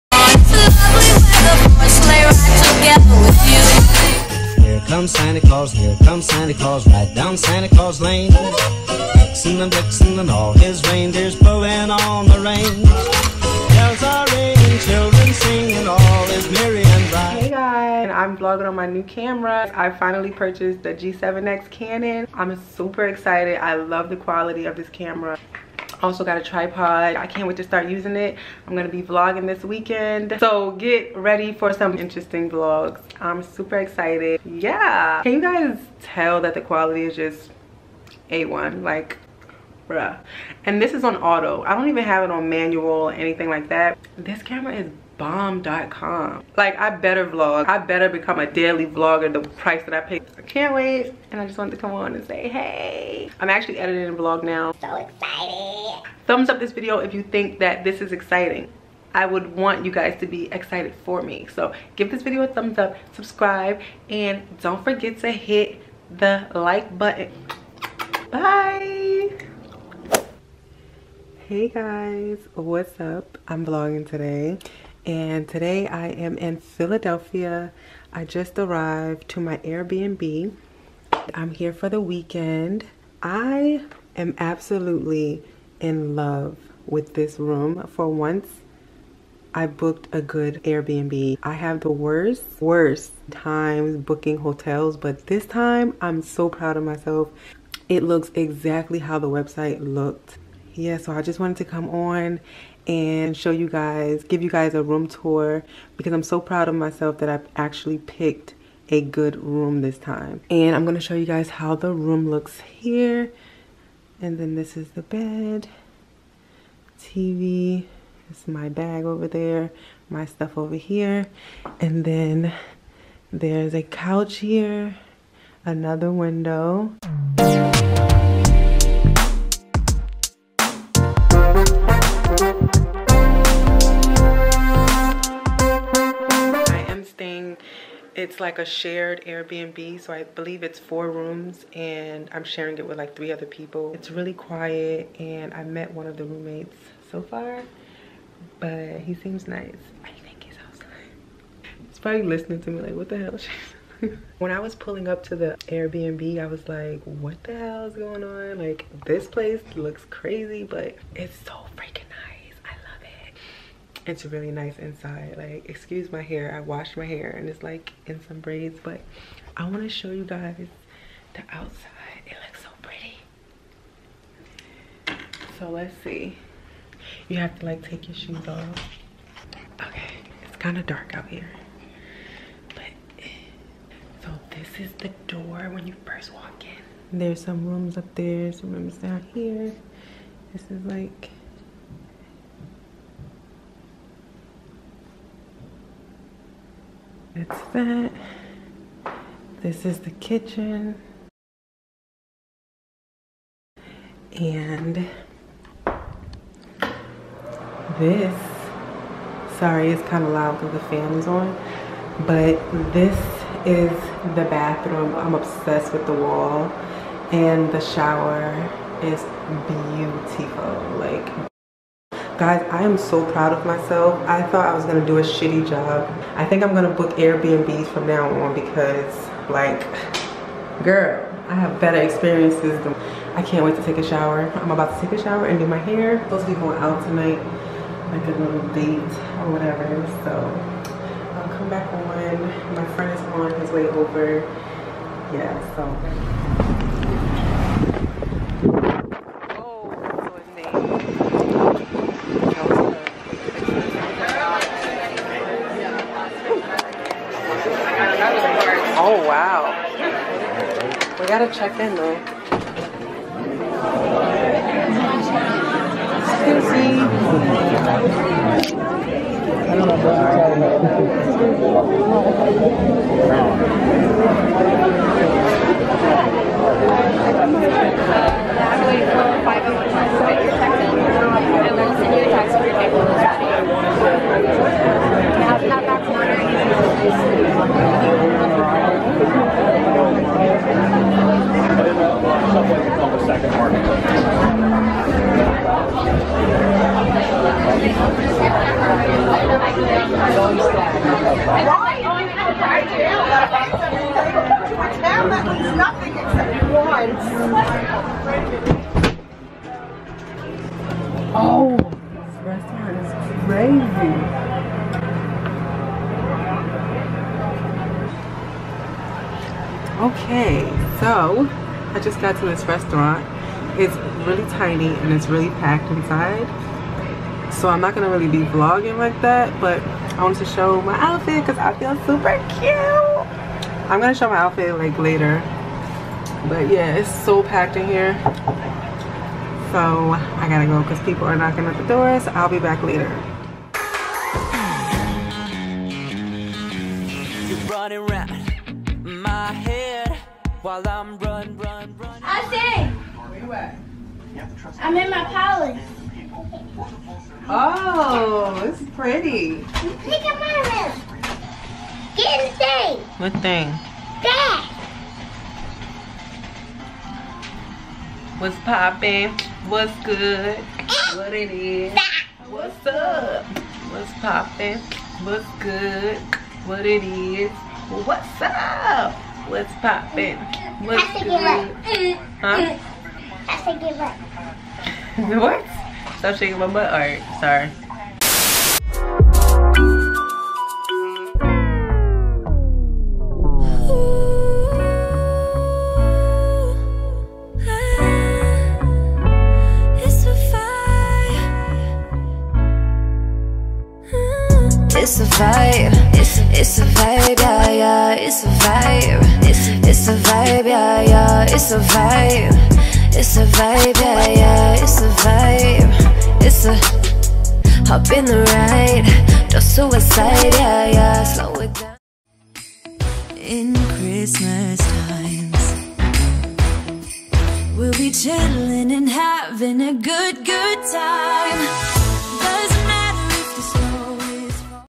Dope Air BNB, new vlog camera cannon G7x and going to Philly with bae
Hey y'all I'm back and I'm finally holding my self accountable to vlogging everyday I'm so excited to participate this year in vlogmas, I hope that your as excited as I am, If  you are then start by giving this video a thumbs up.